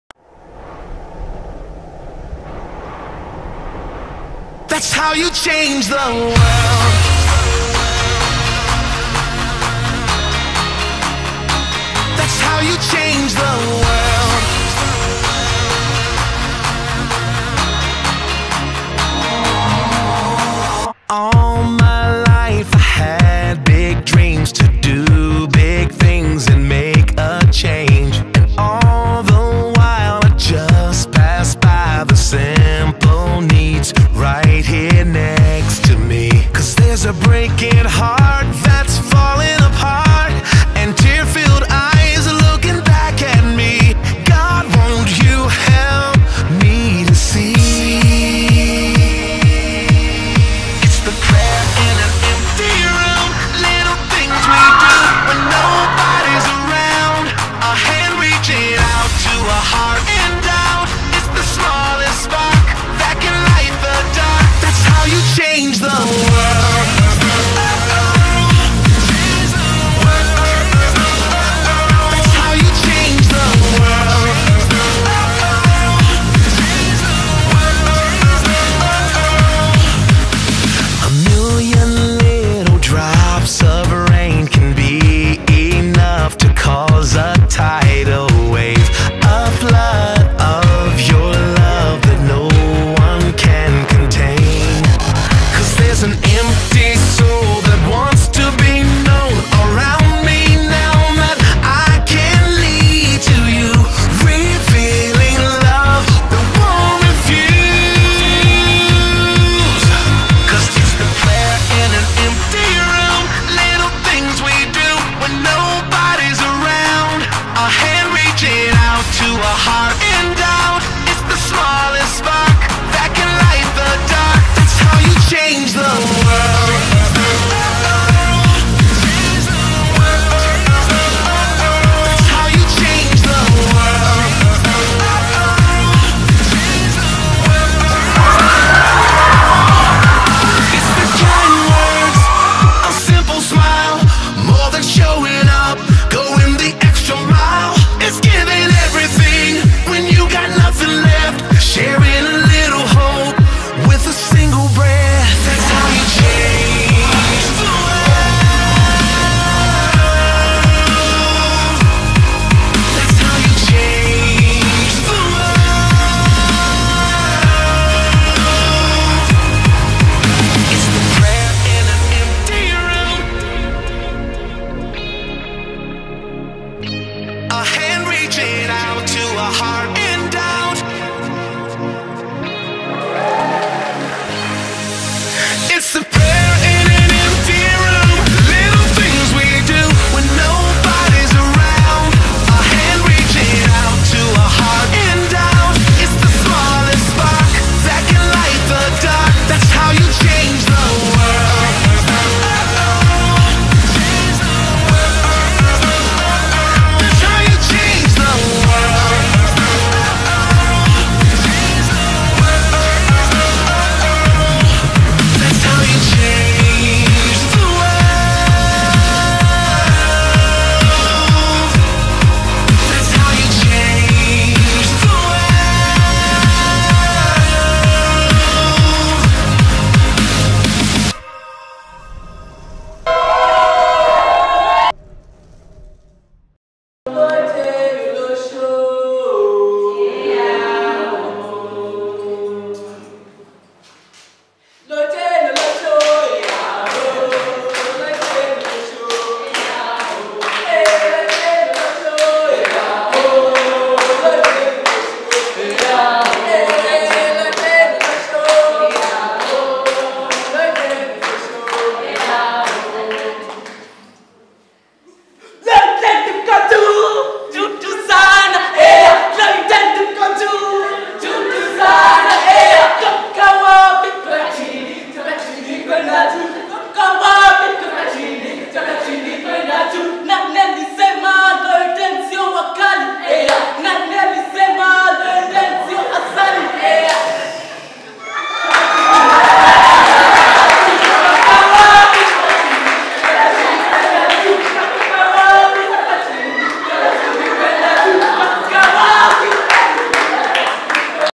Ascent XI class of 2017
the eleventh season of the Ascent leadership experience in Kenyatta university. we at the Journey congratulate this cohort for the amazing strides made.